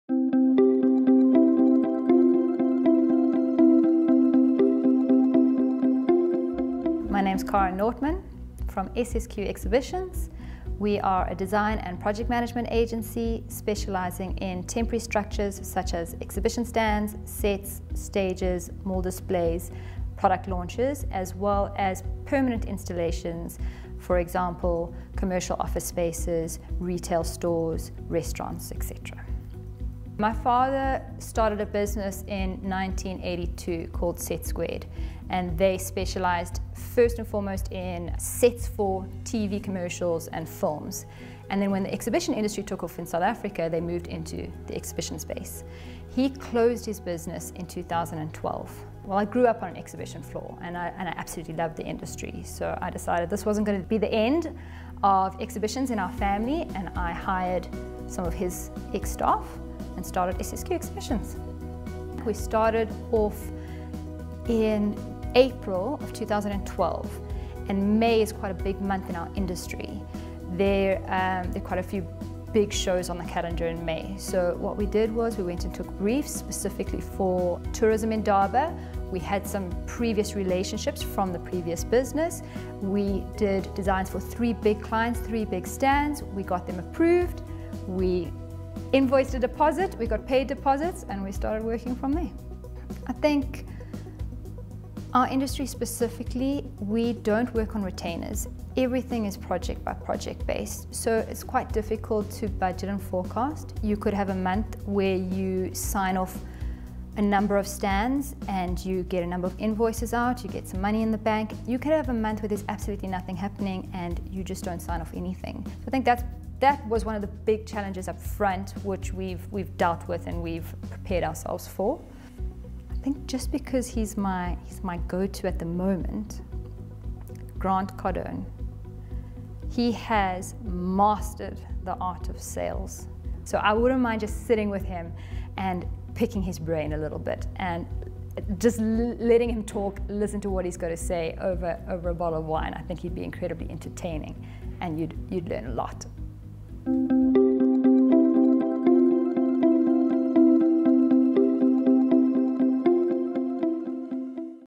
Cara Nortman: SSQ Exhibitions
Prior to establishing SSQ Exhibitions in 2012, Cara Nortman had been working for her father’s company, Set Squared, in the same industry — namely, exhibitions and events. After just two years in the business, Cara made the bold decision to step up to the plate and continue her father’s legacy when he decided to close shop.

While Cara had only been with her father’s company for a short period, she had practically grown up in the industry, having watched her father in the game, and knew with certainty that this is what she wanted to do. When her father made the decision to close his business, Cara approached two members of his then staff and, just like that, SSQ Exhibitions was born.